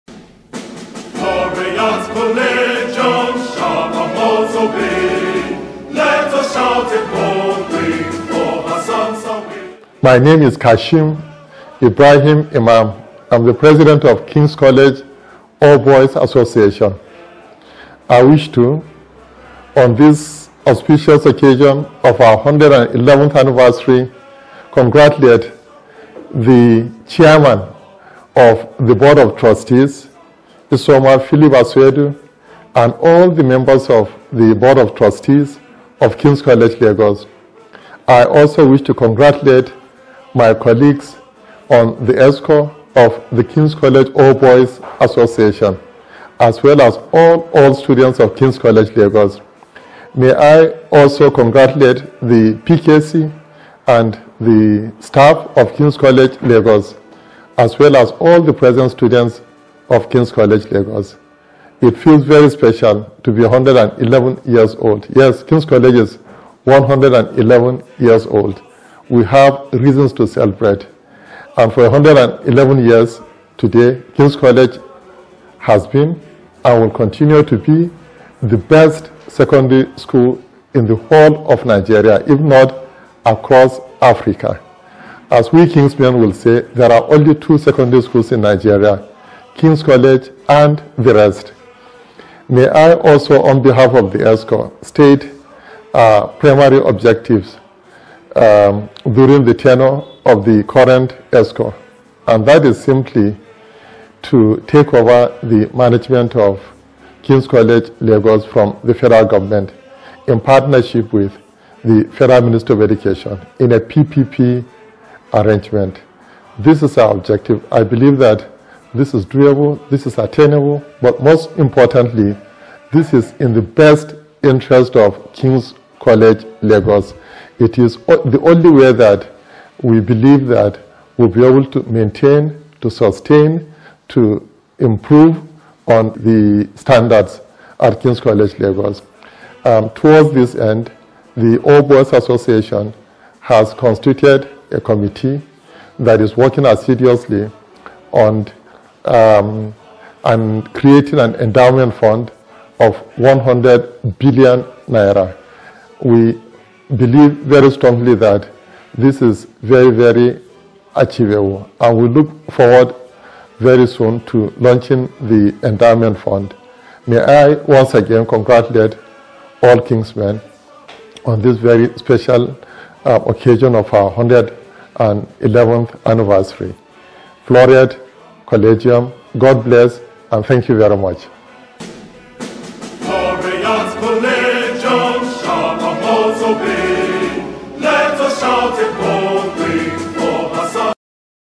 Kashim Ibrahim Imam - KingsWeek 2020 - KCOBA President
Alhaji  Kashim Ibrahim Imam delivers remarks on the 111th Founders Day, KingsWeek 2020